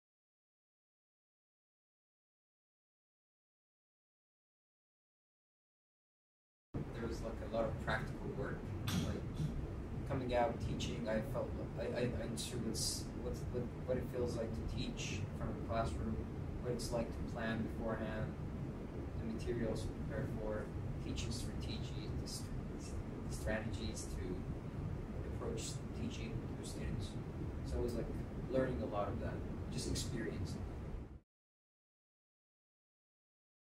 CELTA - An invaluable teaching experience
During the CELTA course, you have the opportunity to do lesson planning and teaching in front of a class. It gives you a full opportunity to practise what you've learnt in the course and receive feedback for it as well.

if you are interested to find out more about our CELTA course here at the British Council.